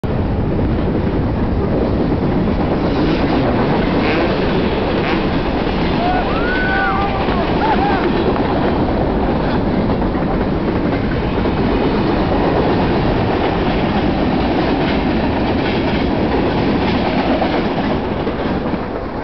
Colin Morrison train jump on a dirtbike in Simi Valley California
Dug up old videos from around 8-9 years ago.